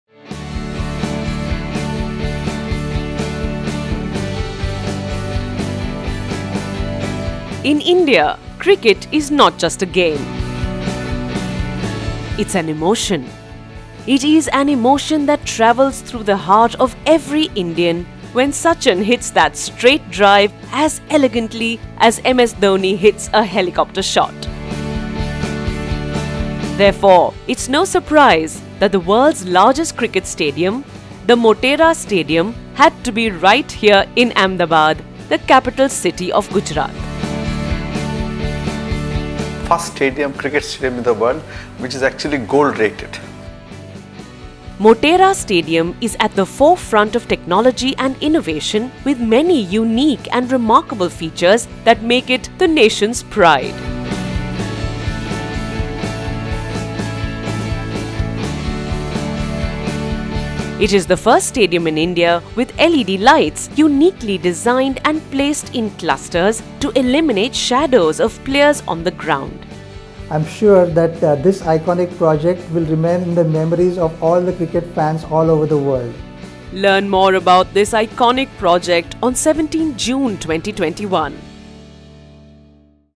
Narendra Modi/Motera Stadium - World's Biggest Cricket Stadium | Construction World Project Teaser
In February 2020, Ahmedabad, the capital city of Gujrat, welcomed the world’s largest cricket stadium to be ever built, the Narendra Modi Stadium or popularly known as the Motera Stadium. The project is a collaboration between the  Gujrat Cricket association and some world-renowned companies. L&T was brought on as the principal contractor, Populous and Walter P Moore contributed their expertise in designing the stadium’s roof.

Here is a little glimpse of our CW Project video on Narendra Modi Stadium.

Presented By UltraTech Cement
Powered By Walter P Moore